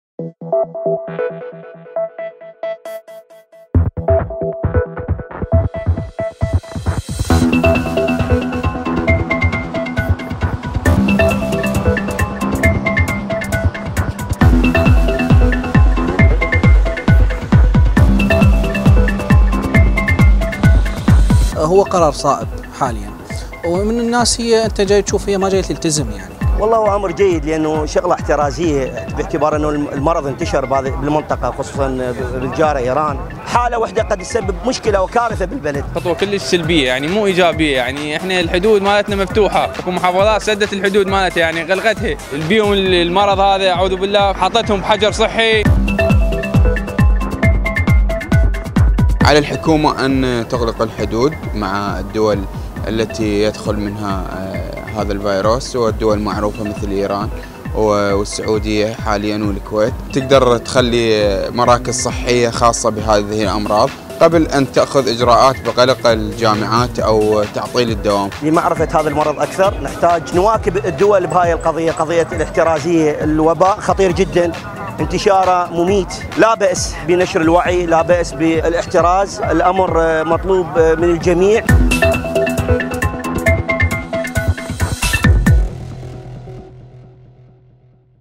الاجراءات للحد من فايروس كورونا
اتخذت الحكومة العراقية مجموعة من الاجراءات للحد من انتشار فايروس كورونا, ما رأي الشارع العراقي بهذه الاجراءات